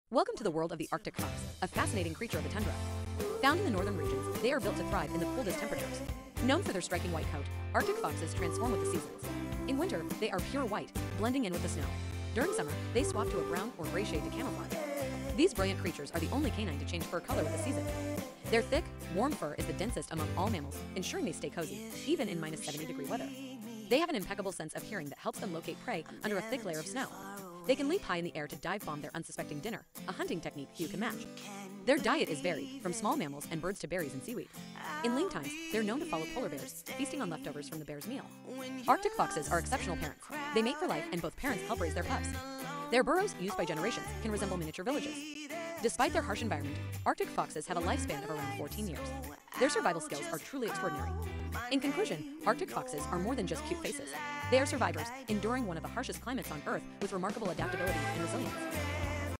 Arctic Fox Magic: Tiny Survivors of the Icy North!
Meet the Arctic Fox, a small and mighty survivor thriving in the icy realms of the North! 🦊❄️ With its magical white fur and incredible adaptations, the Arctic Fox is a true winter wonder. Join us for a quick and frosty adventure to learn amazing facts about these enchanting creatures!